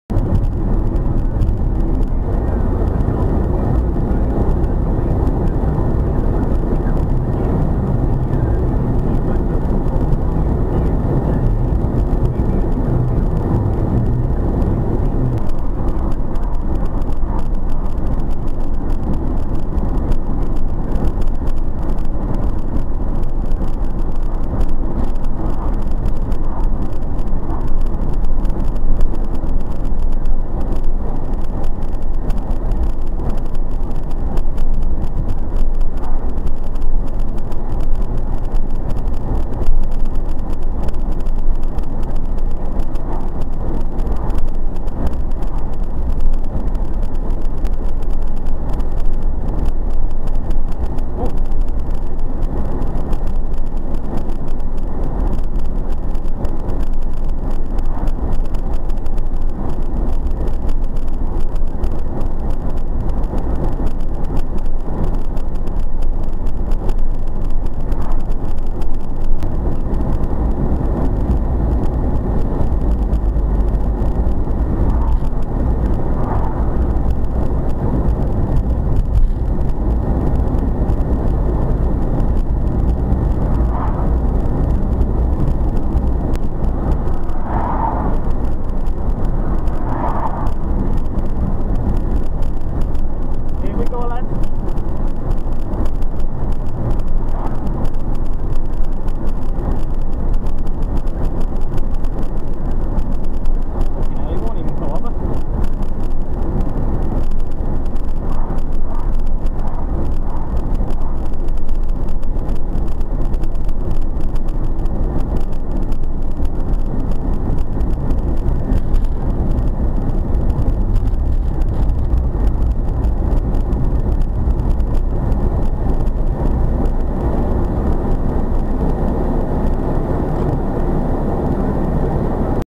Cumbria Dasham - Drunk Driver on A66 2 police arrest him
Part 2 of Drunk driver caught on my dash cam on the A66 in Cumbria. We'd rang the police before this to report him, and in this one he gets pulled, but only after driving passed the police officer trying to pull him in.